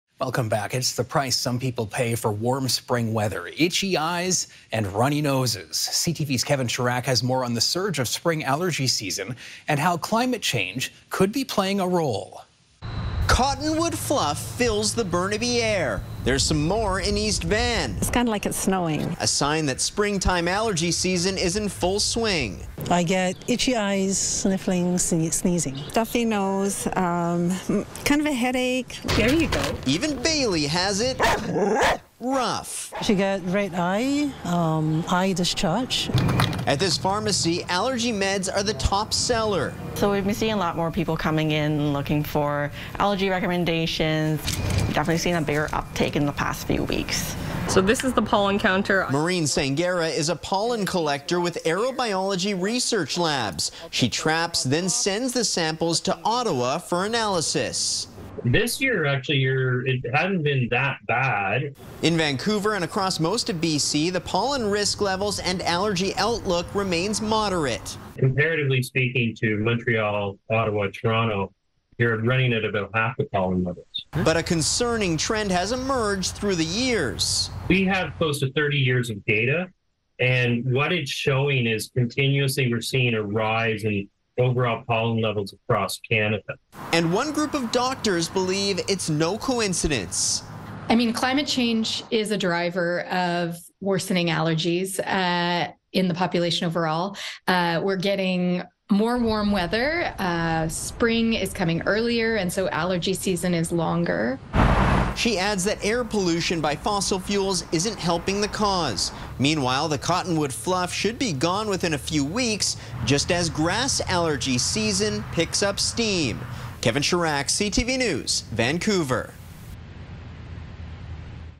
Climate change worsening allergies
Some doctors say runny noses and itchy eyes are becoming more common as the allergy season stretches longer due to climate change.